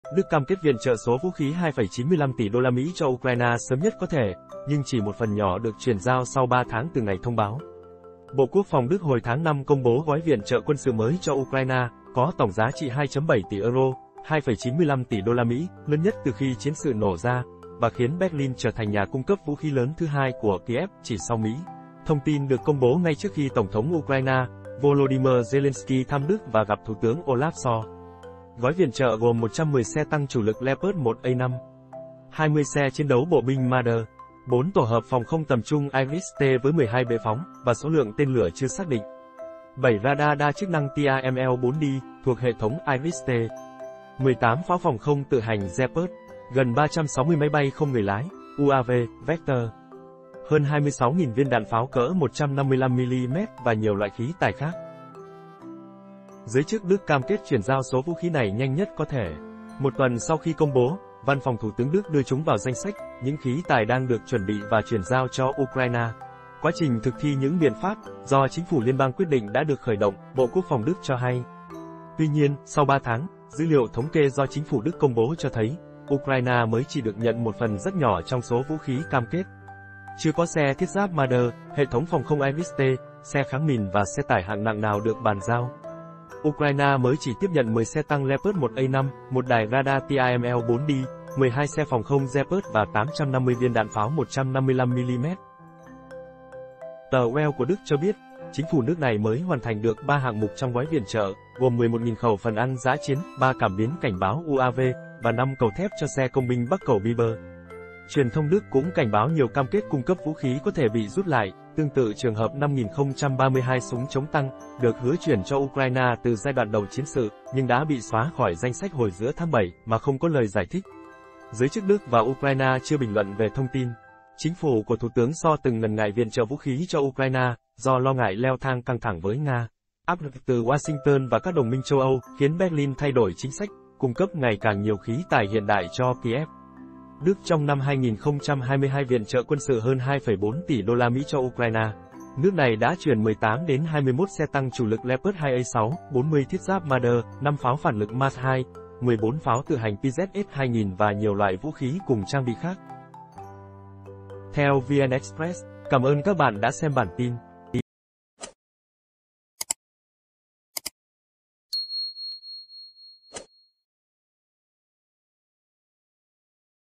TRONG KHI UKRAINE RẤT CẦN VŨ KHÍ THÌ ĐẾN NAY LÔ VŨ KHÍ GẦN 3 TỶ USD CỦA ĐỨC MỚI CHỈ GIAO 1 PHẦN NHỎ
Bộ Quốc phòng Đức hồi tháng 5 công bố gói viện trợ quân sự mới cho Ukraine, có tổng giá trị 2.7 tỷ euro (2.95 tỷ USD), lớn nhất từ khi chiến sự nổ ra, và khiến Berlin trở thành nhà cung cấp vũ khí lớn thứ hai của Kiev, chỉ sau Mỹ. Thông tin được công bố ngay trước khi Tổng thống Ukraine, Volodymyr Zelensky thăm Đức và gặp Thủ tướng Olaf Scholz.